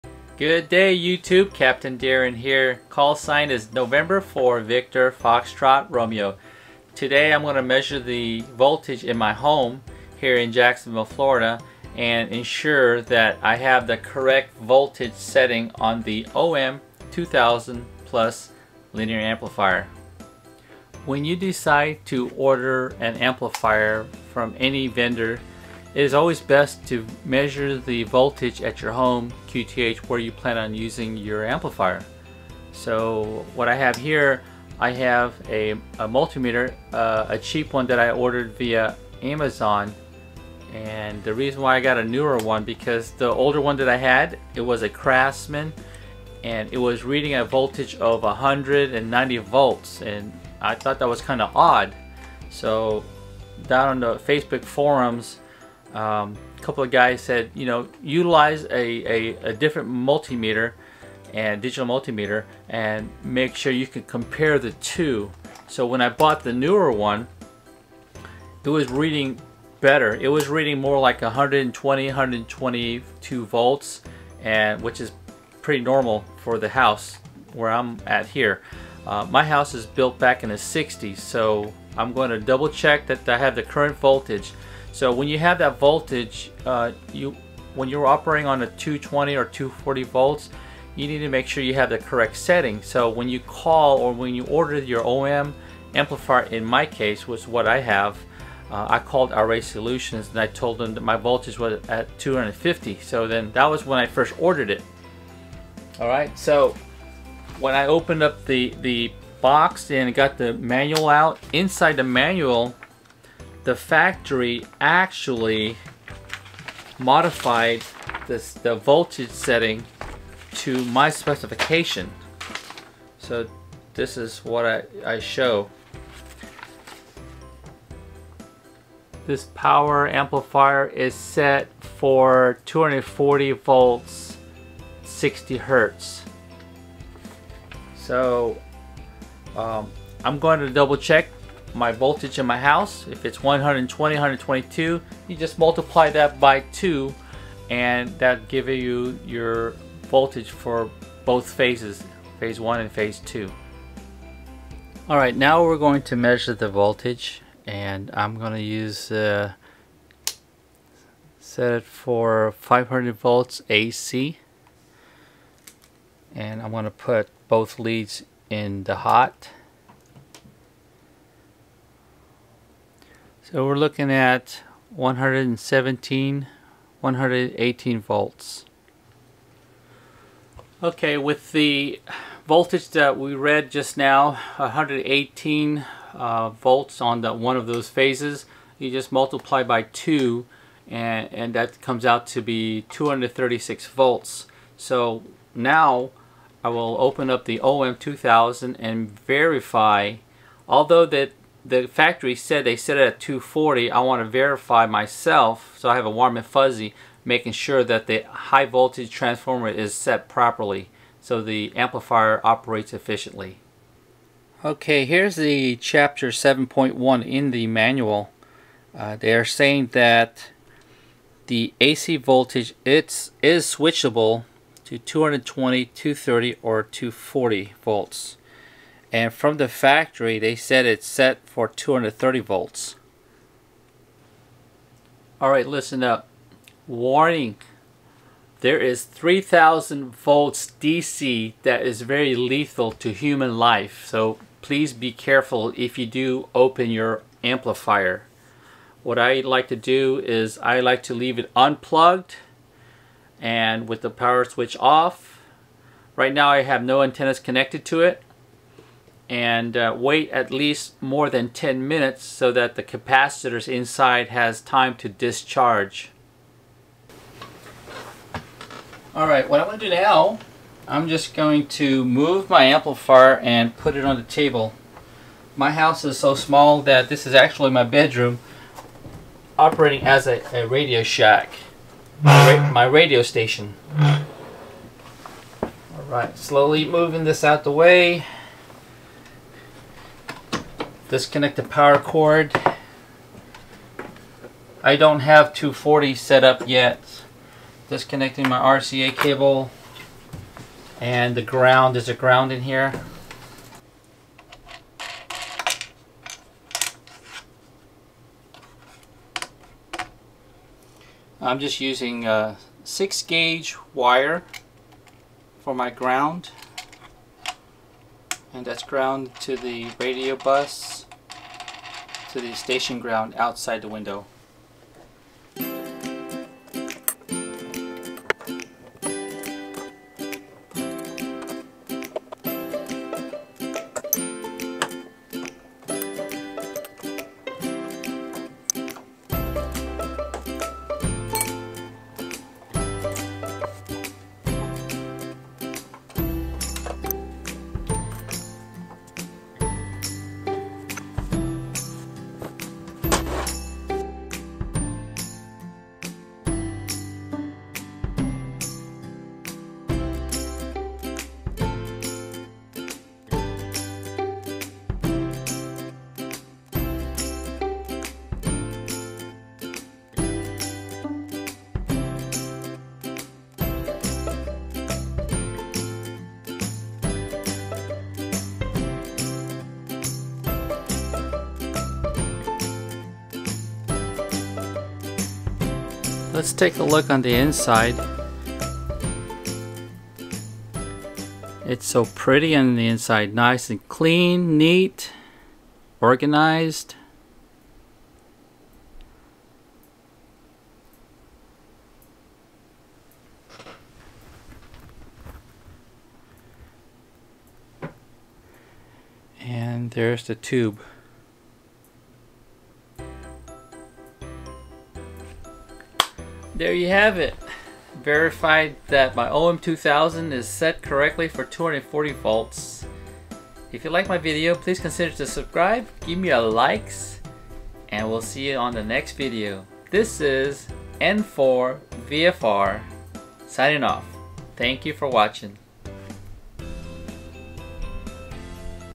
OM2000+ Verify 240V is set
Today, I measured my AC power to verify the readings and to also verify that my OM2000+ HF Amplifier is properly set to 240V. I still need to contact an Electrician to upgrade my home's service panel and also route a 240V outlet in my Ham Shack.

Video Contents
00:00  Intro
00:20  Measure the voltage in your home
02:34  Measuring the power outlet voltage
03:34  Manual state you can switch voltage
03:54  Warnings before you open the Amp
04:27  Opening the Amp
05:59  The inside is beautiful
06:55  Outro